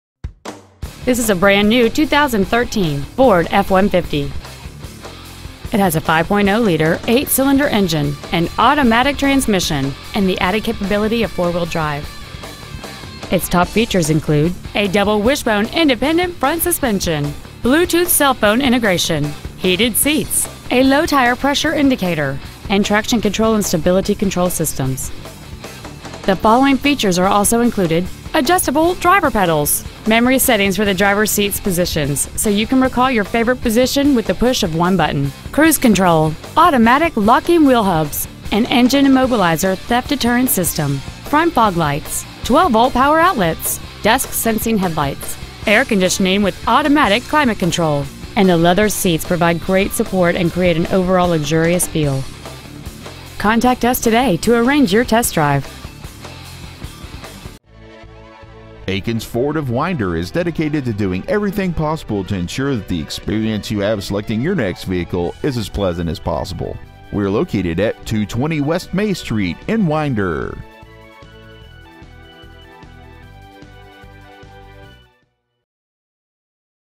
2013 FORD F-150 Winder GA
Year : 2013
 Make : FORD
 Model : F-150
 Engine : 5.0L V8 32V MPFI DOHC FLEXIBLE FUEL
 Trans . : 6-SPEED AUTOMATIC
 Exterior : WHITE PLATINUM METALLIC TRI-COAT
 Miles : 9
 Interior : EBONY
 Stock : DFA44972